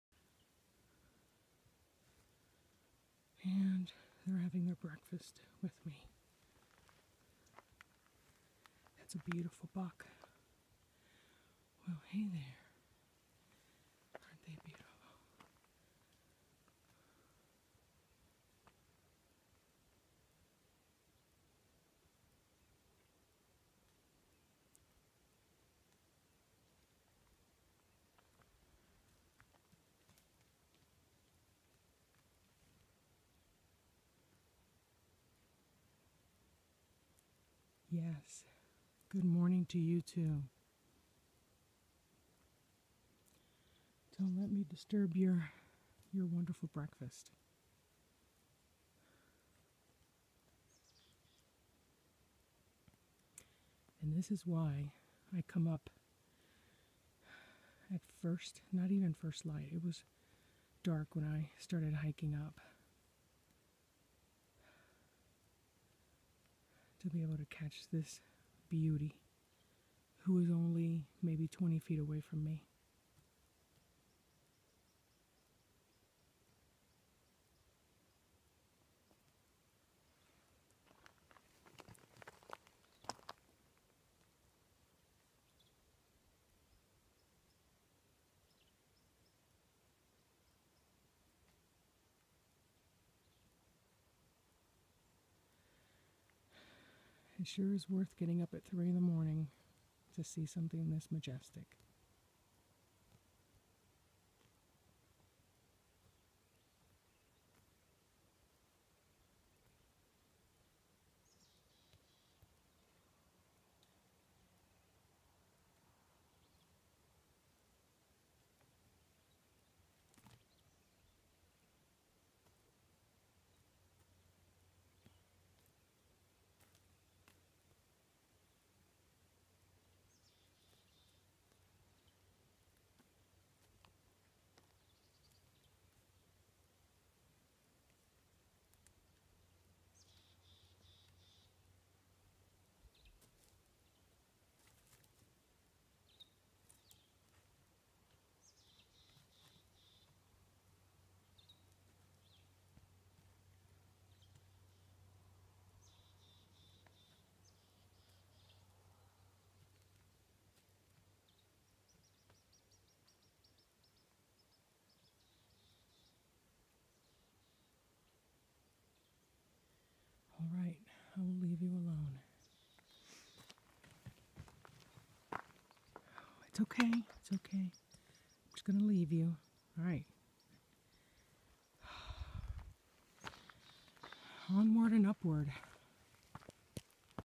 Surprise Mule Deer on North Loop trail
Mule deer on Mt Charleston North Loop to Raintree Trail
Up Close no fear, eating breakfast